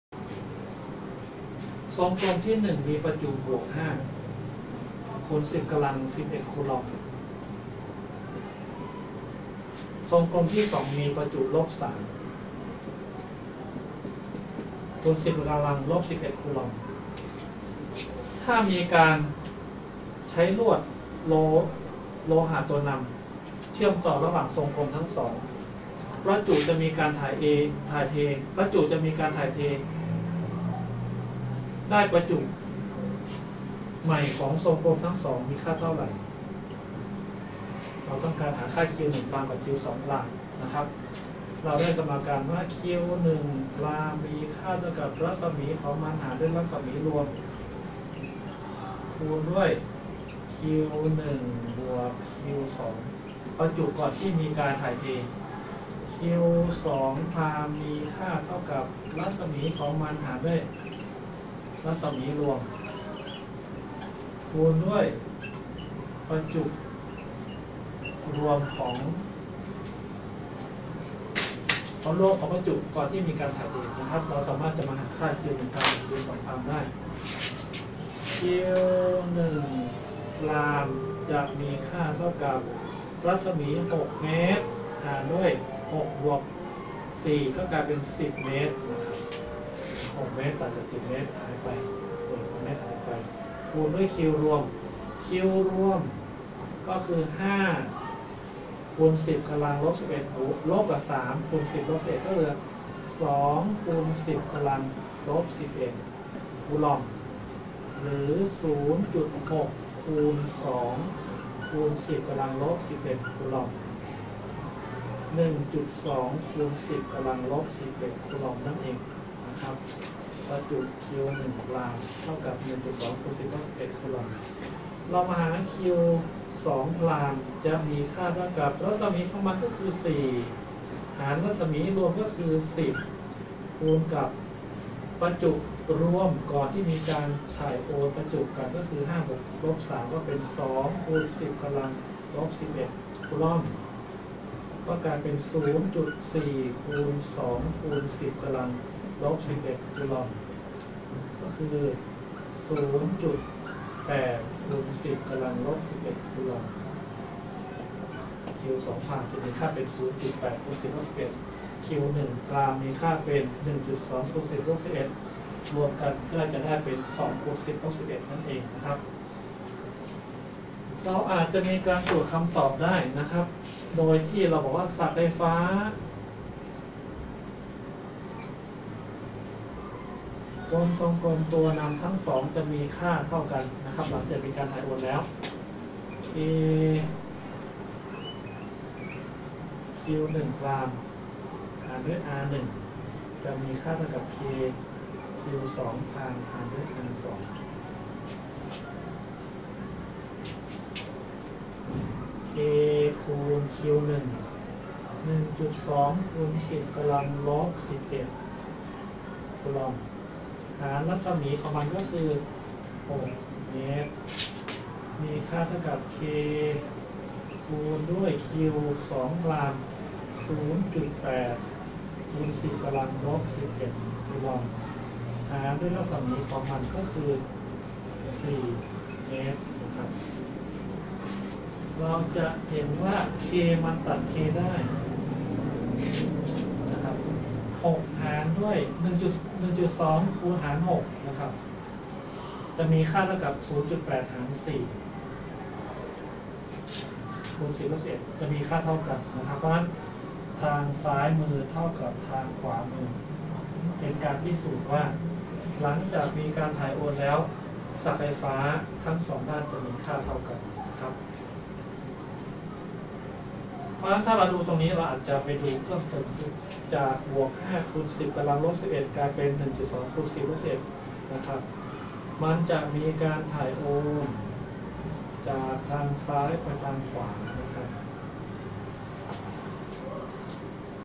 014050 ChargeTransferBetweenSprConductorQ2
Charges transfer between 2 spherical conductors problem.